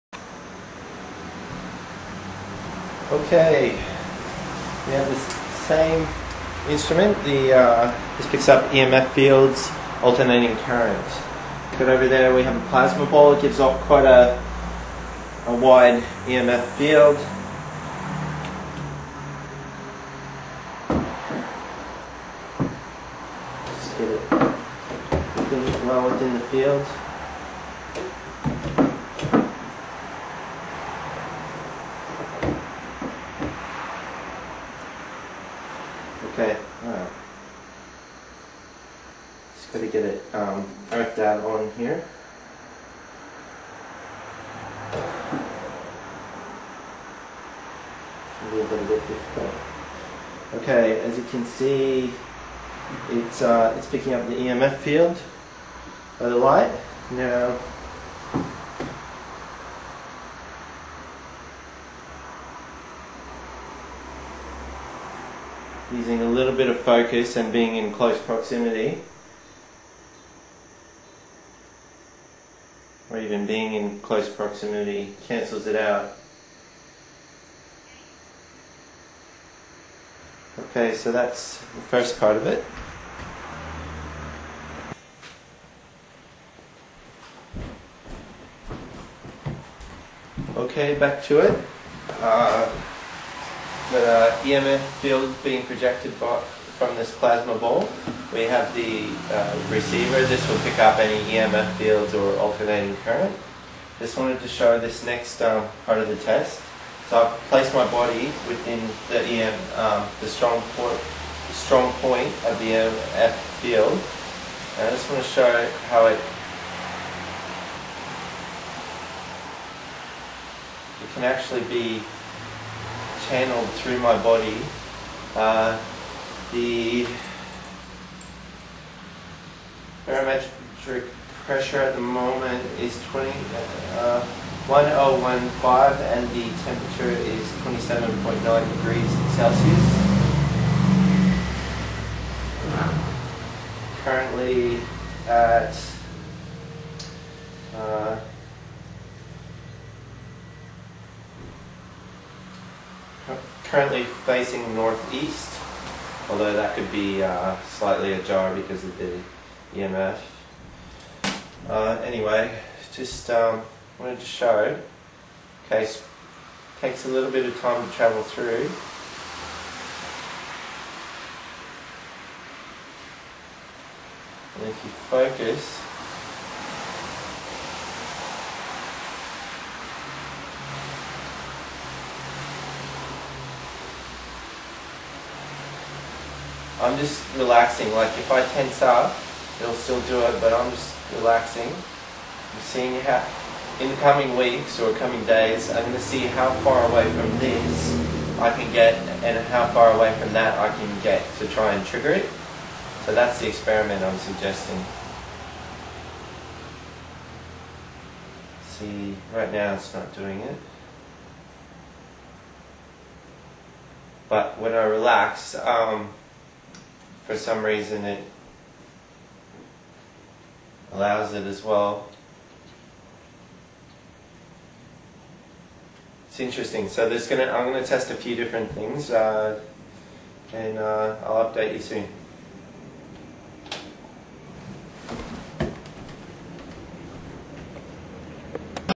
Electrokinesis Emf Experiment 2
This experiment demonstrates. The human bodys ability to both block and ampify electro magnetic fields. Although not clear in the video the objects are not touched which means the actual field is being transmitted. This experiment hopefully will open up new eperimentation avenues for human biofield manipulation of emf. This experiment shows the human body can both block and ampify emf. EMf blocking and channeling potential. Interesting I will post more when I do the next series of tests.
New results
After good sleep and refreshing shower no food no drink.
625am 28.5 c 1012 barometric presure
(Second demonstration Hand 12 cm from plasma ball
other hand 18 cm from emf field pickup. so a 30 cm projection/channeling of the emf field 120 cm total distance to plasma ball.
22 cm emf field interuption effect when trying to stop the emf field (first demonstration)
6:23 pm distance of emf measured from plasma ball no interference at 1.02 metres Temp 27.9 Barometric pressure 1009 hpa. Interference range of bio emf 74 cm from emf reader.